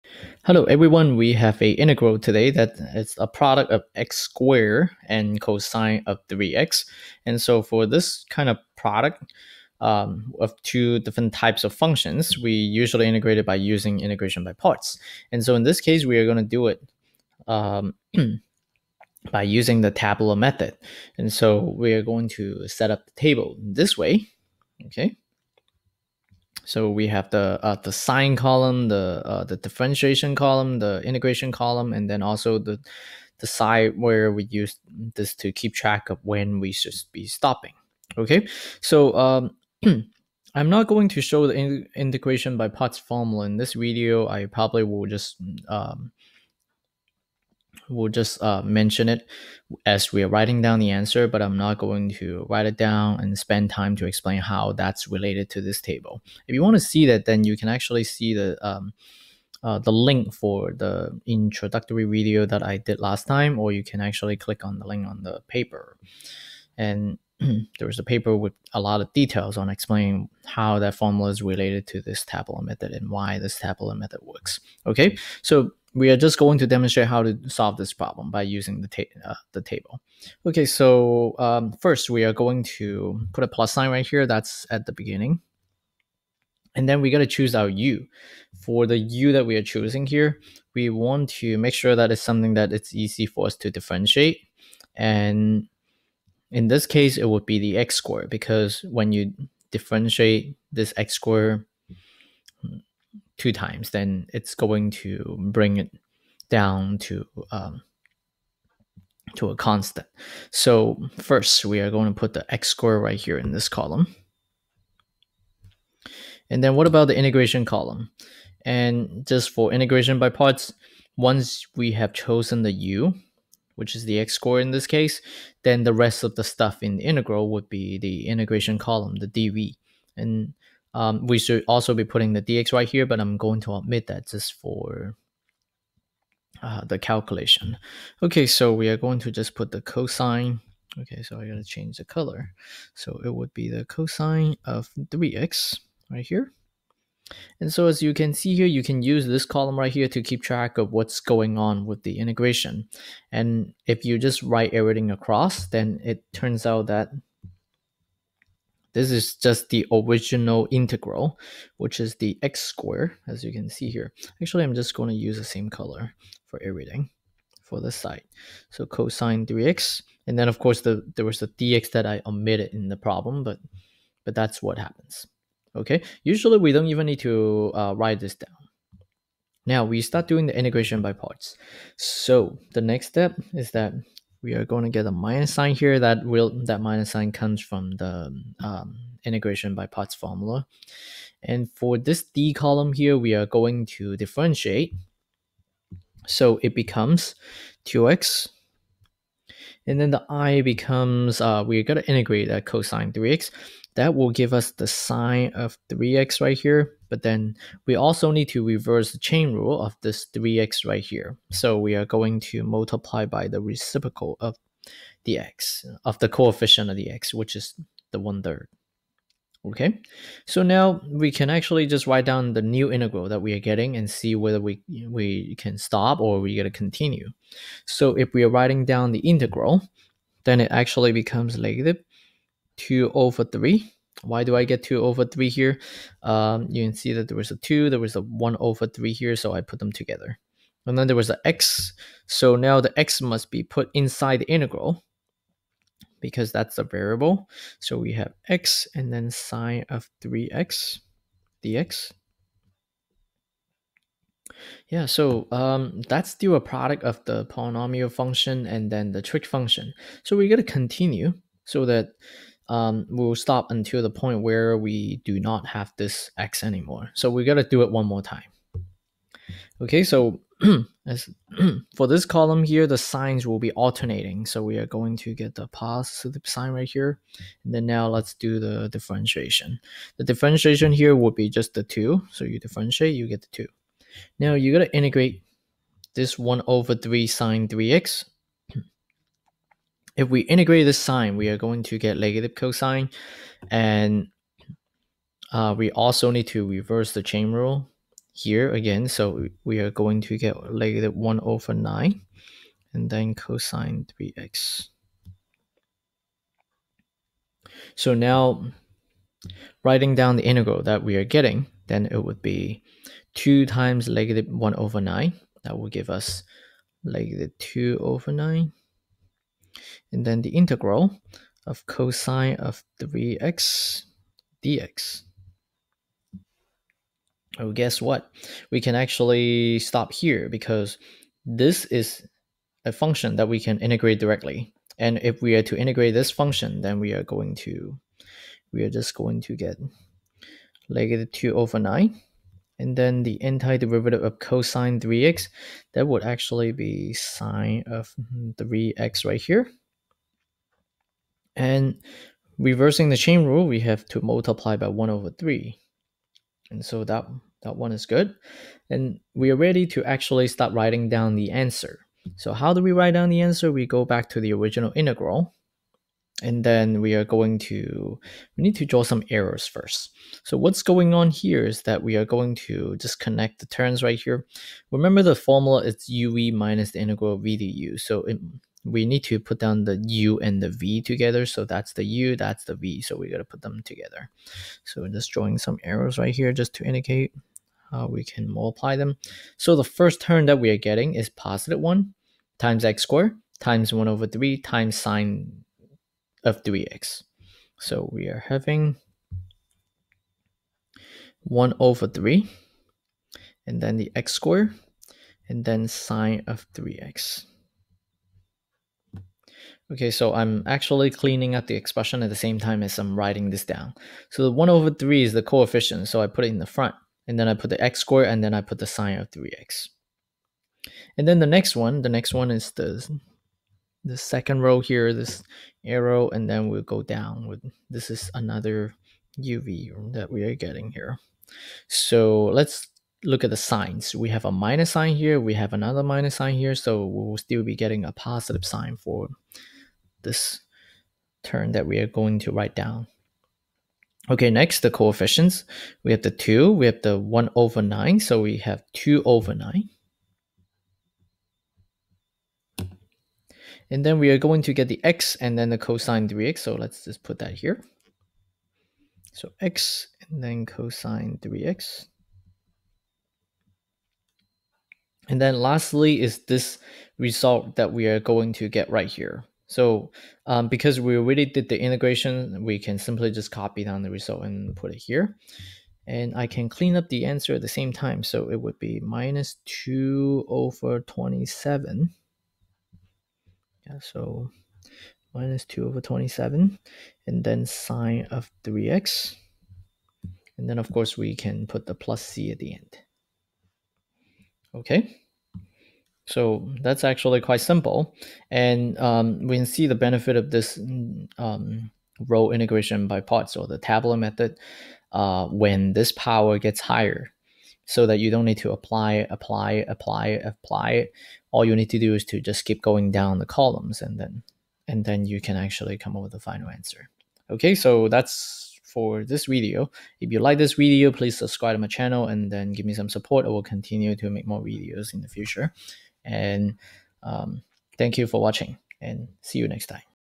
Integration by Parts of Power and Trig Function x^2 cos(3x) by Tabular Method | Glass of Numbers
Integration by parts is fun! It's more fun and easy when you use the row integration by parts (RIP, Tabular Method).

In this vidoe, we integrate a product of a power of x and a trig function by parts. Since the whole number power of x will go lower when differentiated, we use let u = the power function and dv = trig function.

See how the problem is done using the traditional method of integration by parts: